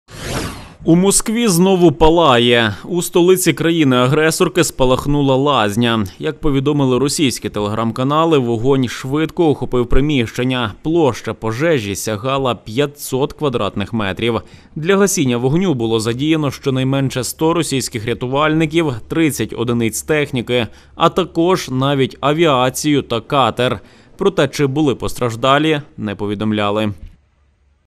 🔥москва ПАЛАЄ… Спалахнула лазня: пожежа НАСТІЛЬКИ ПОТУЖНА, що задіяли навіть авіацію і катер
У москві знову палає! У столиці країни-агресорки спалахнула лазня.
Як повідомили російські телеграм-канали, вогонь шивдко охопив приміщення, площа пожежі сягала 500 м.кв.
Для гасіння вогню було задіяно щонайменше 100 російських рятувальників, 30 одиниць техніки  а також навіть авіацію та катер